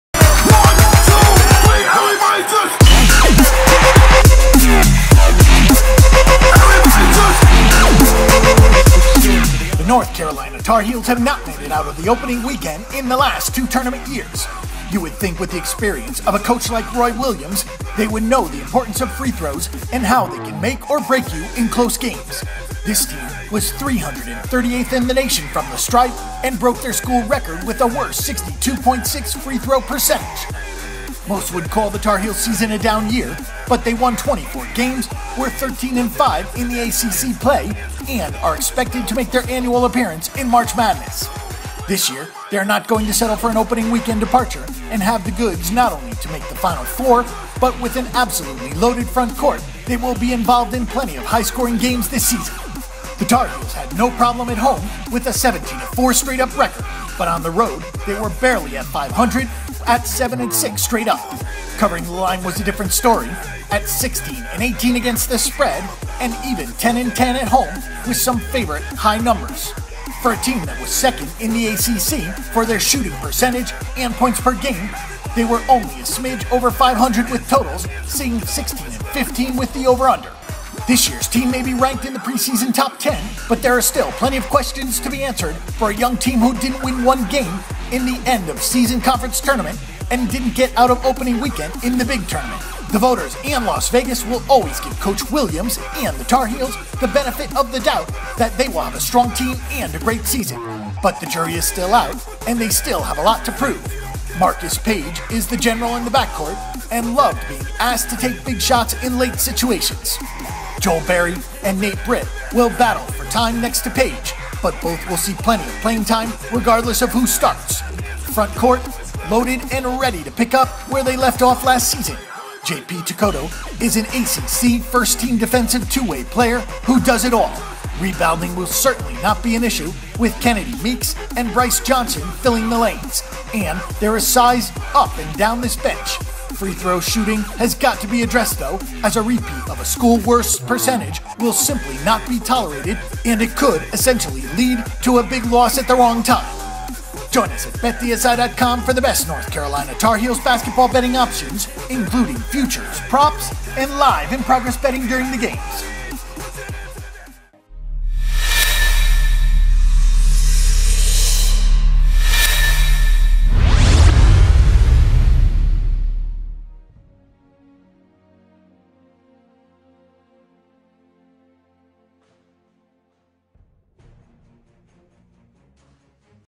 North Carolina Tarheels Odds | 2014-2015 NCAA Basketball Team Betting Preview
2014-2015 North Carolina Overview

This year's team may be ranked in the pre-season top 10, but there are still plenty of questions to be answered for a young team who didn't win one game in their end of season Conference tournament and didn't get out of opening weekend in the big tournament. The voters and Las Vegas will always give Coach WIlliams and the Heels the benefit of the doubt that they will have a strong team and a great season, but the jury is still out and they still have a lot to prove. Marcus Paige is the General in the backcourt and loved being asked to take big shots in late situations.

Joel Berry and Nate Britt will battle for time next to Paige, but both with see plenty of playing time regardless of who starts. Front court? Loaded and ready to pick up where they left off. J.P Tokoto is an ACC first team defensive two-way player who does it all. Rebounding will certainly not be an issue with Kennedy Meeks and Brice Johnson filling the lanes, and there is size up and down this bench. Free throw shooting has got to be addressed, as a repeat of a school worst % will simply not be acceptable and it could essentially lead to a big loss at the wrong time.